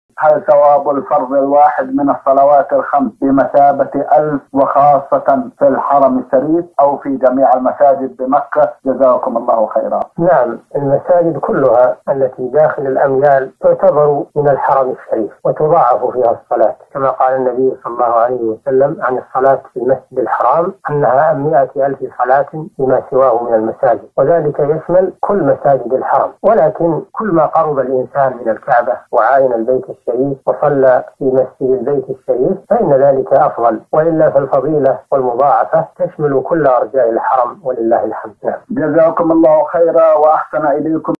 هل ثواب الفرض الواحد من الصلوات الخمس بمثابة ألف وخاصة في الحرم الشريف أو في جميع المساجد بمكة؟
نص السؤال:
هل ثواب الفرض الواحد من الصلوات الخمس بمثابة ألف وخاصة في الحرم الشريف أو في جميع المساجد بمكة جزاكم الله خيراً؟
السيرة الذاتية :
للاشتراك في قناة الشيخ صالح الفوزان

رئيس الفريق
لمشروع كبار العلماء